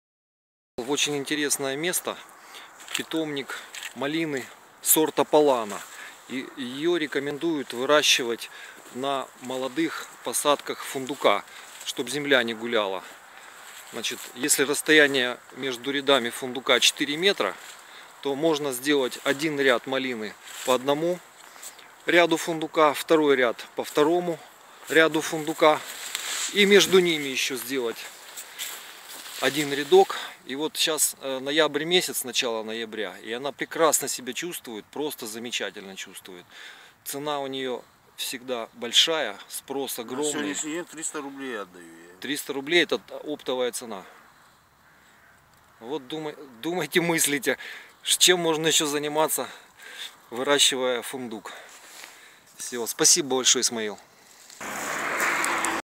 Фундук Трапезунд.Малина как уплотнитель молодого орешника.
Познакомился с Исмаилом-открытым человеком,увлеченным фундуком.Фундук начинает плодоносить на третий,четвертый год.Промышленный урожай дает на шестой,седьмой год.ВСЕ ЭТО ВРЕМЯ МОЖНО ПОЛУЧАТЬ ПРИБЫЛЬ С МАЛИНЫ!Два часа мы общались на эту тему,голова кругом.Первый урожай Исмаил получает от древовидной малины,забыл,то ли конец мая то ли начало июня,затем до морозов плодоносит Полана.Если есть вода и навоз-всем рекомендую.